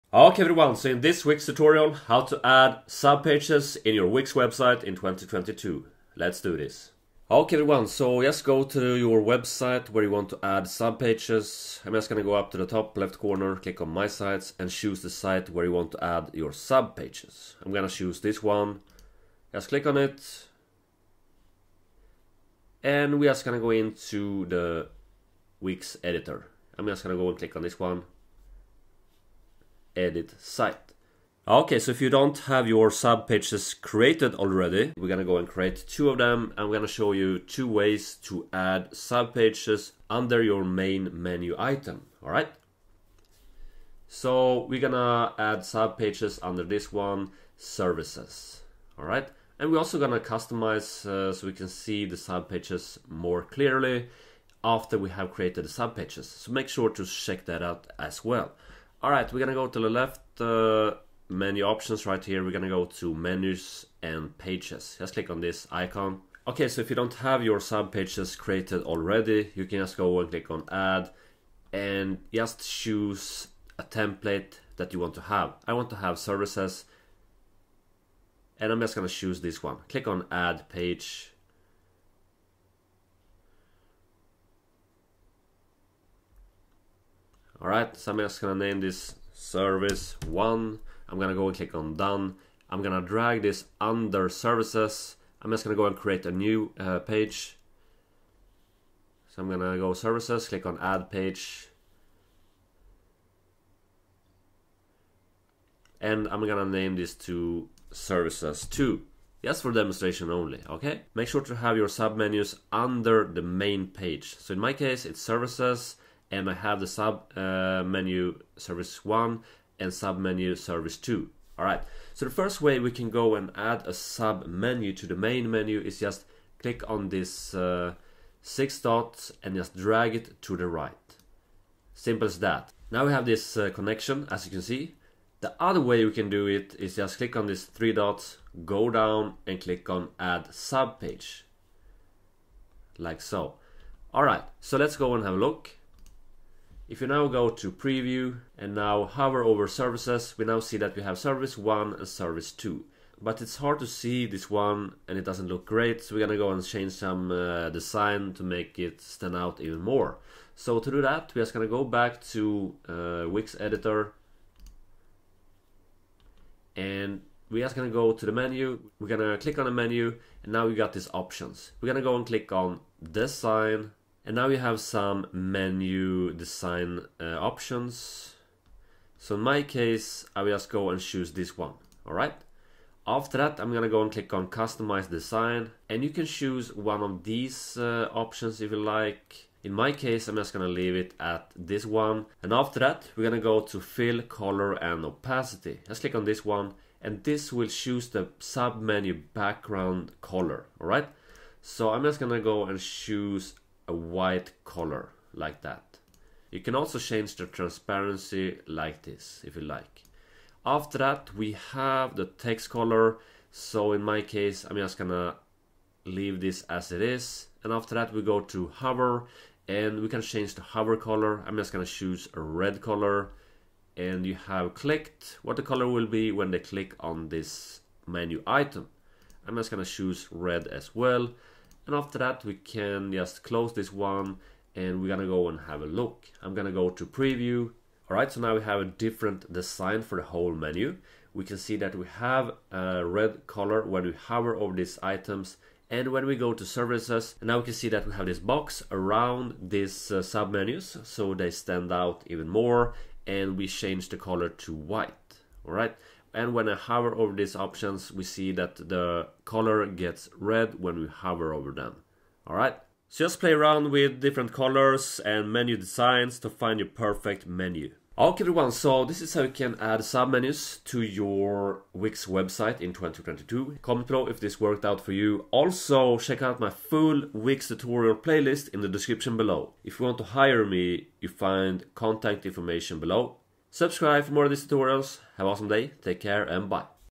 How to Add Subpages in Wix Website in 2022
How to easily add subpages to your menu on your Wix website in 2022.

I will show you 2 different ways to create a subpage under the main menu item. We will also have a lock at how you can customize your menu and subpages with different colors and designs.